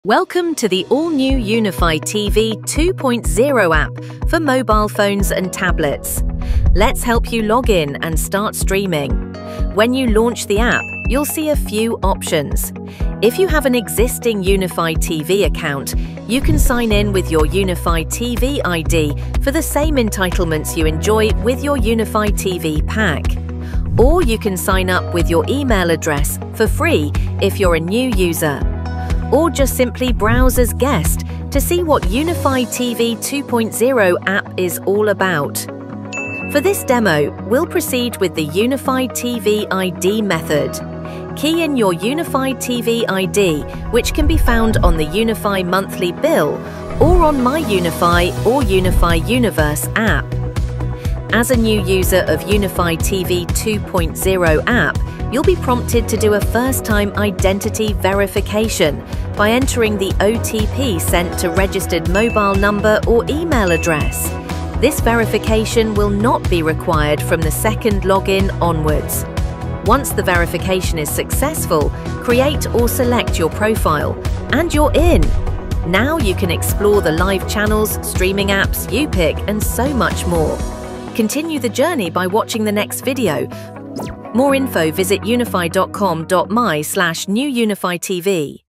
HOW TO LOGIN TO UNIFI TV 2.0 APP | MOBILE PHONE & TABLET​
Here’s how to log in to the latest Unifi TV 2.0 app on mobile devices. Watch your favourite content on the go — anytime, anywhere!​